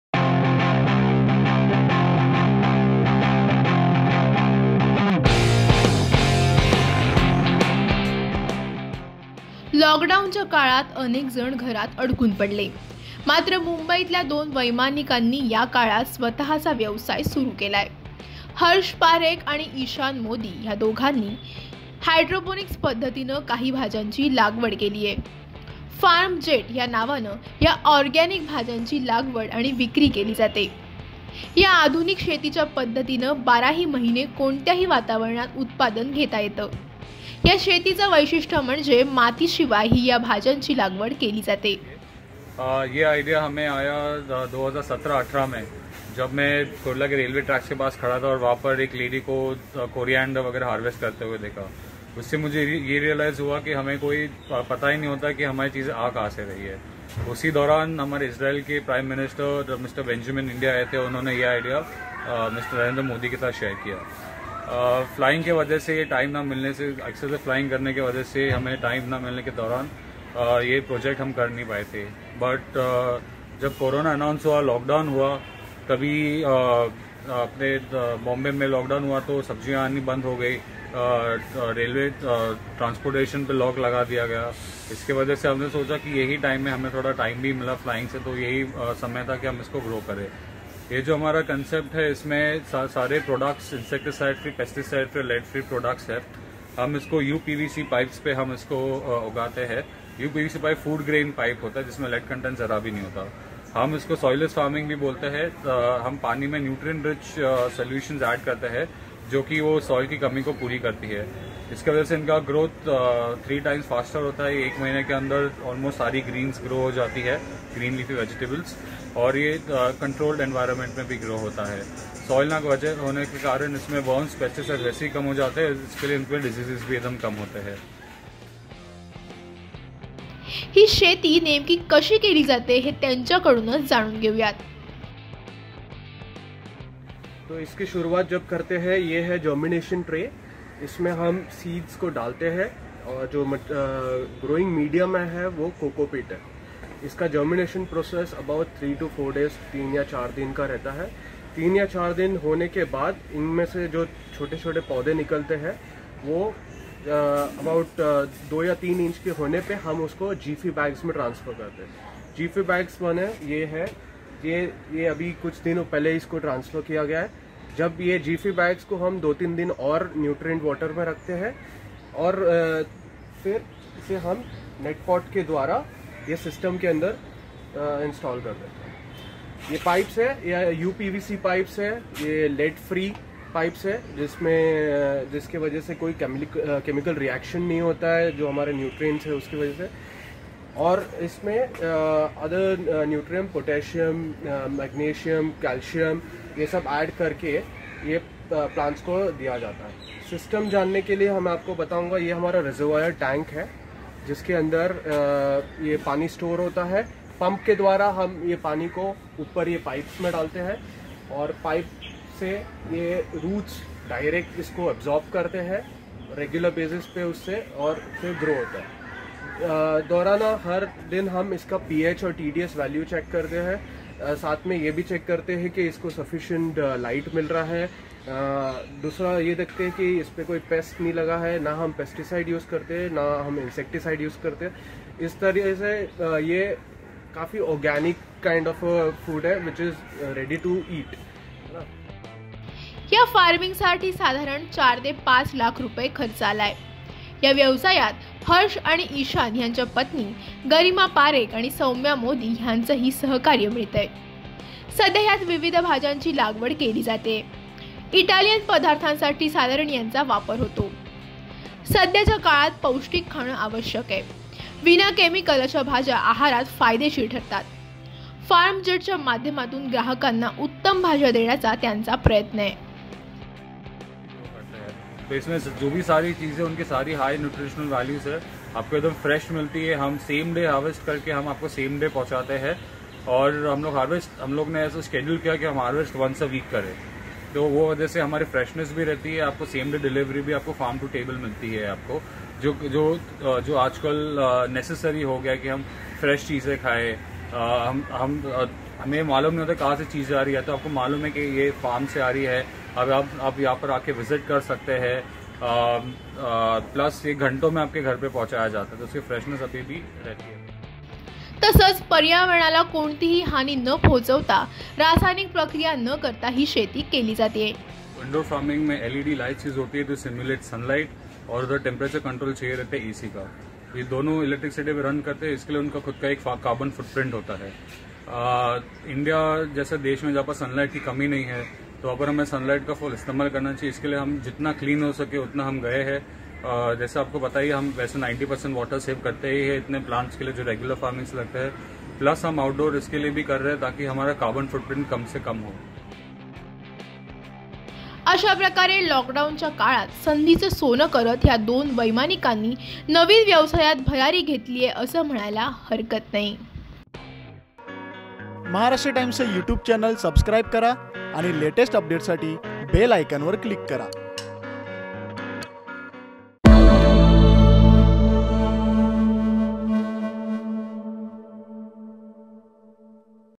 लॉकडाउनच्या काळात वैमानिक वळले आधुनिक शेतीकडे | Pilots growing vegetables using new technology
लॉकडाउनच्या काळात अनेकजण घरात अडकून पडले
मात्र मुंबईतल्या दोन वैमानिकांनी या काळात स्वत:चा व्यवसाय सुरु केलाय
हर्ष पारेख आणि ईशान मोदी या दोघांनी हायड्रोपोनिक्स पद्धतीने काही भाज्यांची लागवड केली
'फार्म जेट' या नावाने या ऑरगॅनिक भाज्यांची लागवड आणि विक्री केली जाते
या आधुनिक शेतीच्या पद्धतीने १२ महिने कोणत्याही वातावरणात उत्पादन घेता येतं
या शेतीचं वैशिष्ट्य म्हणजे मातीशिवाय ही या भाज्यांची लागवड केली जाते